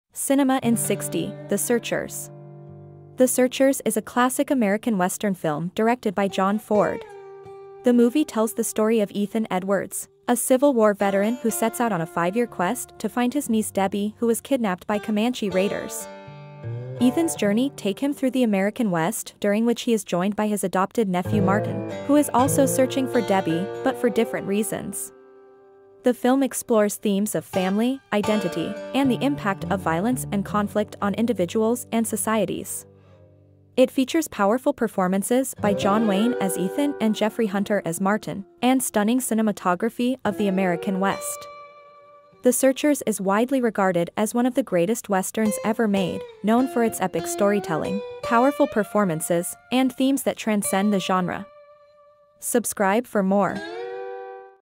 The Searchers: Cinema in 60
Cinema in 60: The Searchers. The Searchers is a classic American western film directed by John Ford. The movie tells the story of Ethan Edwards, a Civil War veteran who sets out on a five-year quest to find his niece Debbie who was kidnapped by Comanche raiders. Ethan's journey take him through the American West during which he is joined by his adopted nephew Martin, who is also searching for Debbie, but for different reasons. The film explores themes of family, identity, and the impact of violence and conflict on individuals and societies. It features powerful performances by John Wayne as Ethan and Jeffrey Hunter as Martin, and stunning cinematography of the American West. The Searchers is widely regarded as one of the greatest westerns ever made, known for its epic storytelling, powerful performances, and themes that transcend the genre.